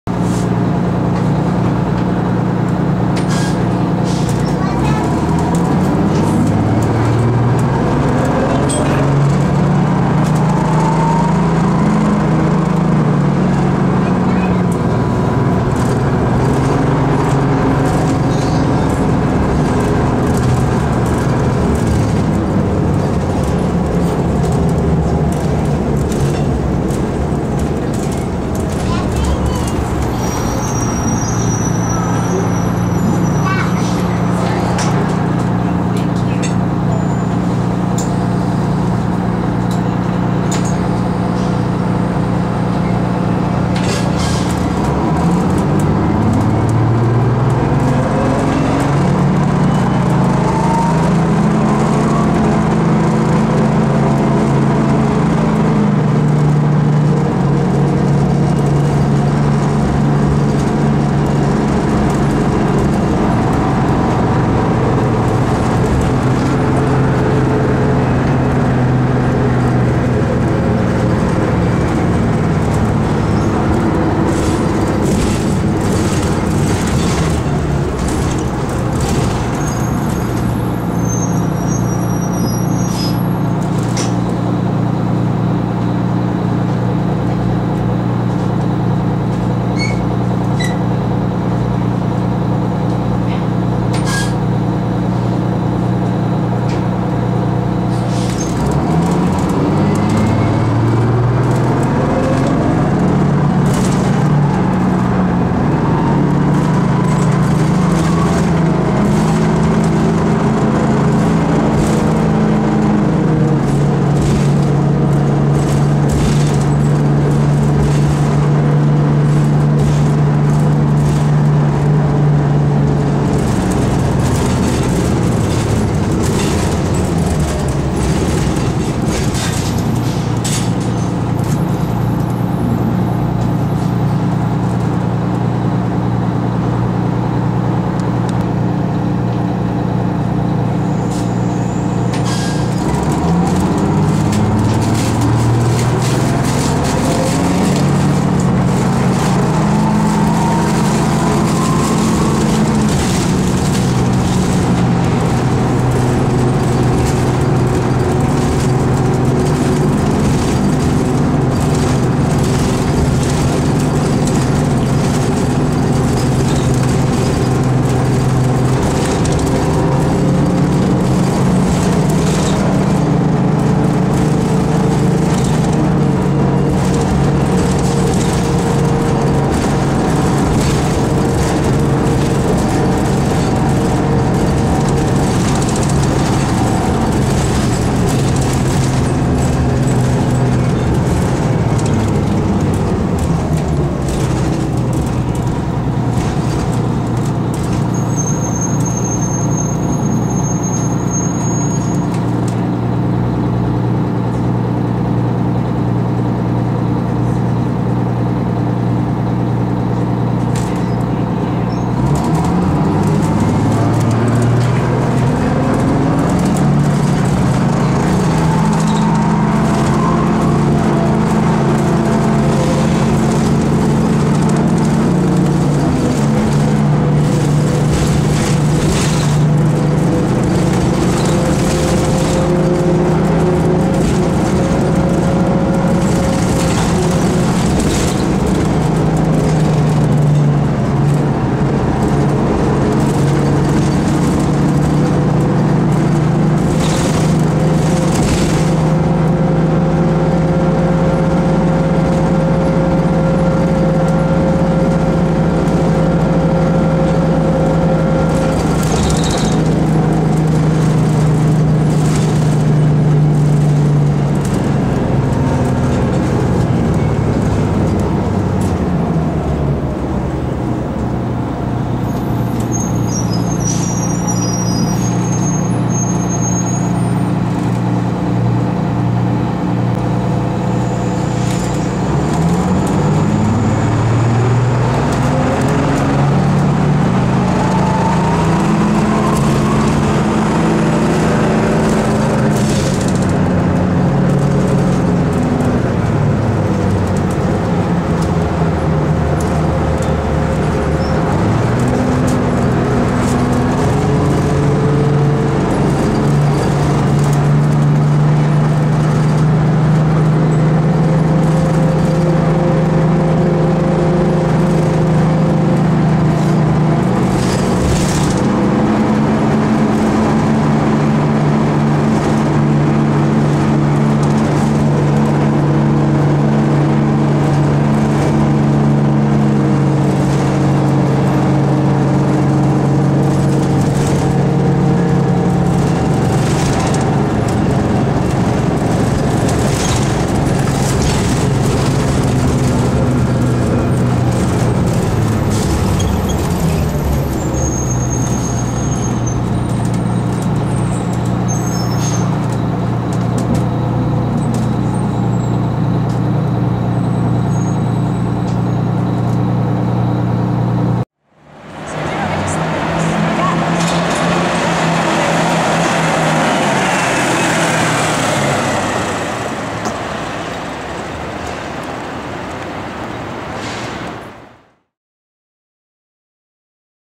Calgary Transit MCI Classic #5018
A rare extra on an otherwise low-floor route. This 1991 bus was on the southbound route 3 from Sandstone terminal.